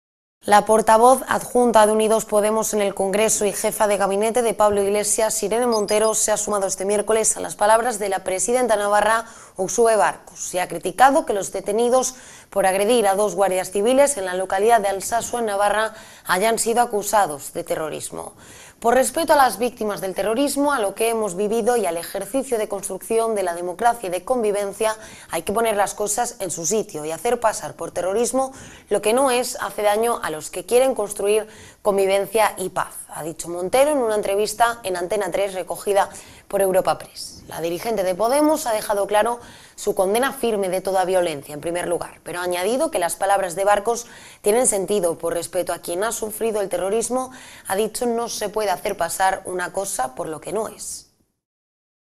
Málaga 24h TV Podemos critica que los agresores de Alsasua sean acusados de terrorismo
La portavoz adjunta de Unidos Podemos en el Congreso y jefa de gabinete de Pablo Iglesias, Irene Montero, se ha sumado este miércoles a las palabras de la presidenta navarra, Uxue Barkos (Geora Bai) y ha criticado que los detenidos por agredir a dos guardias civiles en la localidad de Alsasua hayan sido acusados de terrorismo.

"Por respeto a las víctimas del terrorismo, a lo que hemos vivido y al ejercicio de construcción de la democracia y de convivencia, hay que poner las cosas en su sitio, y hacer pasar por terrorismo lo que no es hace daño a los que quieren construir convivencia y paz", ha dicho Montero en una entrevista en Antena 3, recogida por Europa Press.
La dirigente de Podemos ha dejado claro su "condena firme de toda violencia, en primer lugar" pero ha añadido que las palabras de Barkos tienen sentido. Por respeto a quien ha sufrido el terrorismo, ha dicho, no se puede "hacer pasar una cosa por lo que no es".

English:
The deputy spokeswoman of Unidos Podemos in Congress and head of the cabinet of Pablo Iglesias, Irene Montero, has joined Wednesday the words of the president Navarra, Uxue Barkos (Geora Bai) and has criticized that the arrested for attacking two guards Civilians in the town of Alsasua have been accused of terrorism.

"Out of respect for the victims of terrorism, what we have lived and the exercise of building democracy and coexistence, we must put things in their place, and make terrorism happen what is not doing harm to those who want Build coexistence and peace, "said Montero in an interview on Antena 3, collected by Europa Press.
The Weavers leader has made clear his "firm condemnation of all violence in the first place" but added that Barkos' words make sense. Out of respect for anyone who has suffered terrorism, he said, you can not "make a thing happen for what it is not."